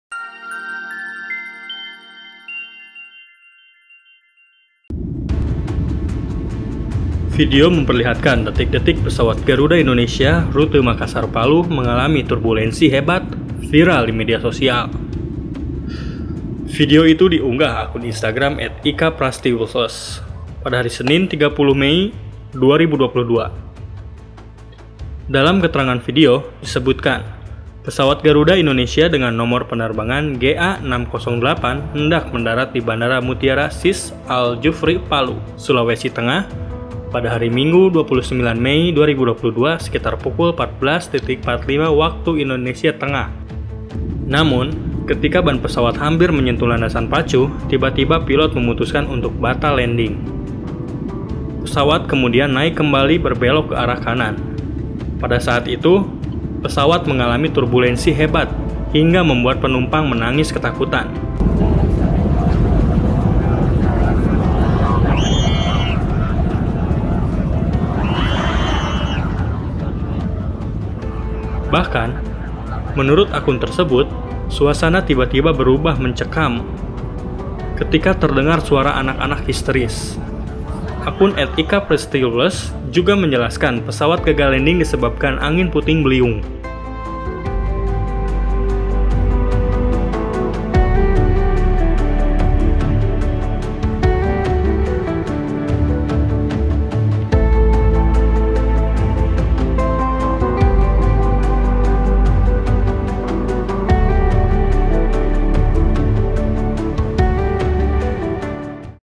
Mencekam! Detik-detik Pesawat Garuda Diterjang Puting Beliung
Video itu diunggah akun Instagram @ikapratiwles, Senin (30/5/2022).

Dalam keterangan video, disebutkan pesawat Garuda Indonesia dengan nomor penerbangan GA 608 hendak mendarat di Bandara Mutiara Sis Aljufri Palu, Sulawesi Tengah, Minggu (29/5/2022) sekitar pukul 14.45 WITA.

Namun ketika ban pesawat hampir menyentuh landasan pacu, tiba-tiba pilot memutuskan untuk batal landing.

Pesawat kemudian naik kembali dan berbelok ke arah kanan.

Pada saat itu, pesawat mengalami turbulensi hebat